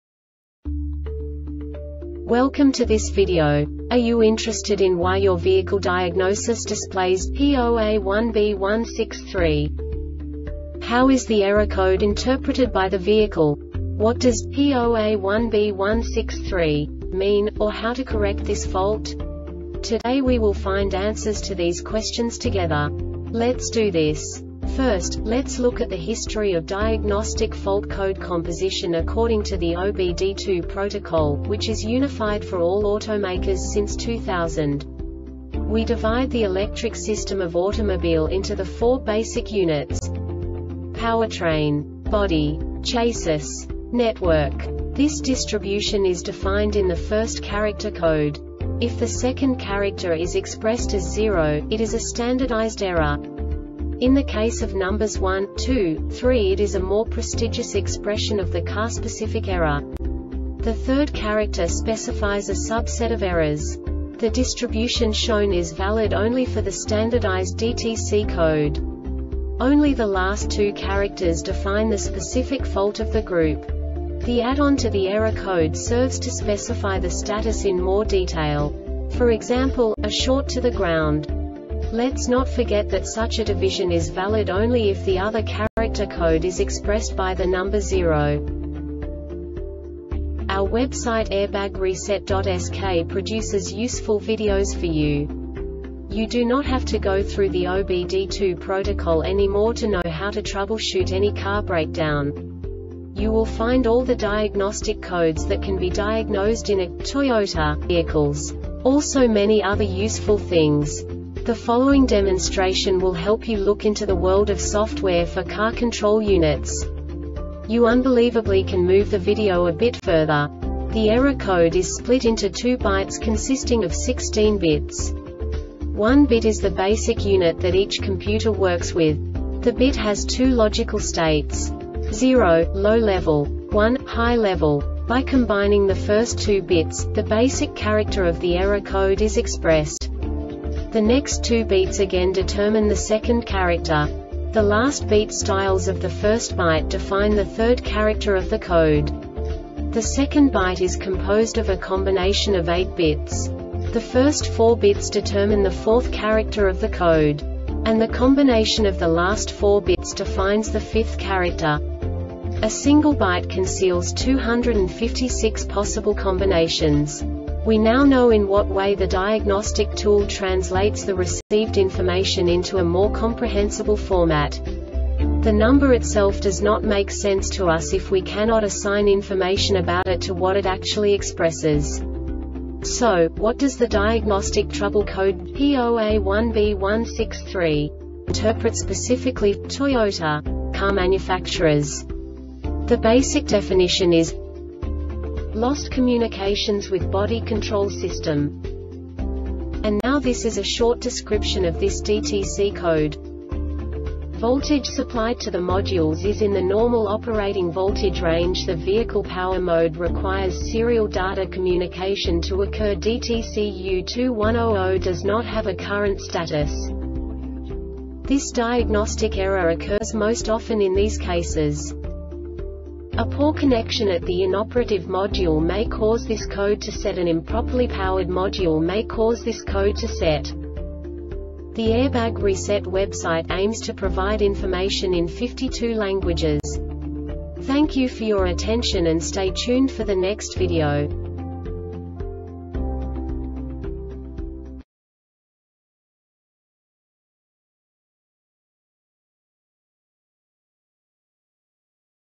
DTC Toyota P0A1B-163 Short Explanation
Contents:
0:21 Basic DTC analysis according to OBD2 protocol standard.
1:48 Insight into programming
2:58 A diagnostically understandable description of the Diagnostic Trouble Code
3:05 Basic error definition

Make:
Toyota
Code:
P0A1B 163
Definition:
Lost Communications With Body Control System
Description:
Voltage supplied to the modules is in the normal operating voltage range.The vehicle power mode requires serial data communication to occur.DTC U2100 does not have a current status.